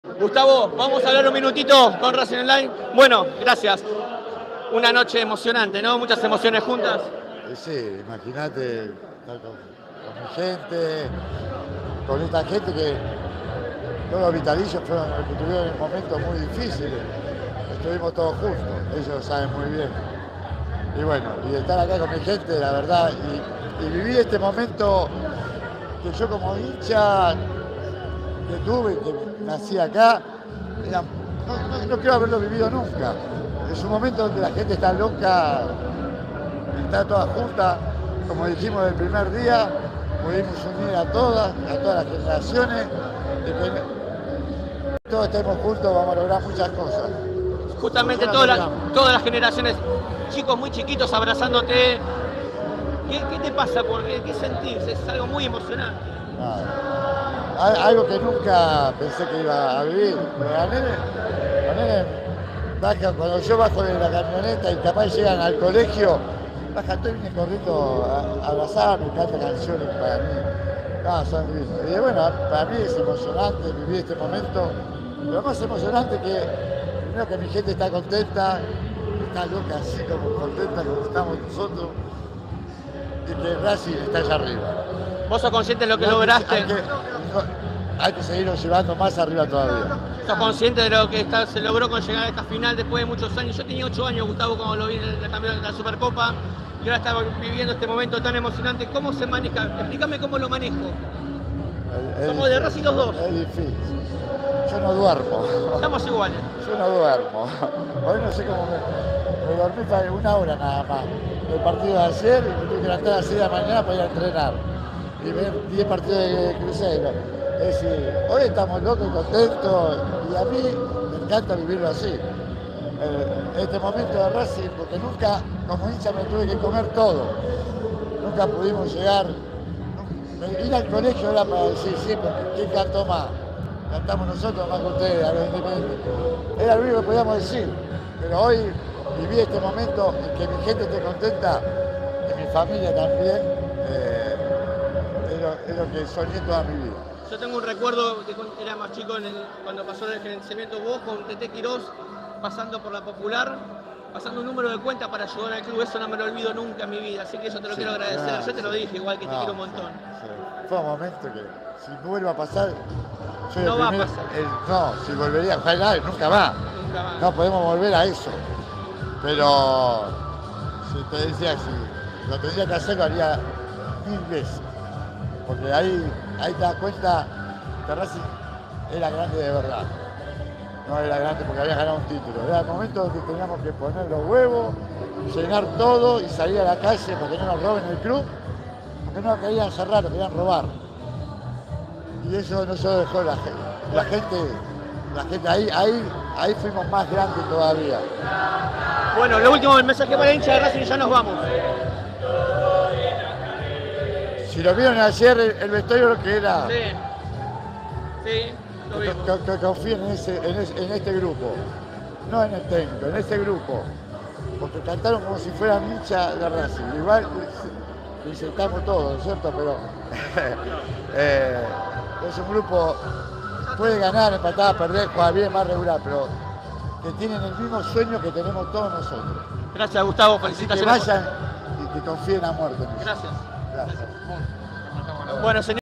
"CONFIEN EN ESTE GRUPO" GUSTAVO COSTAS EN LA PREVIA DE LA FINAL DE LA COPA SUDAMERICANA
RacingOnline, en sintonía con tu pasión. ¡Prendete a la radio de Racing! 📻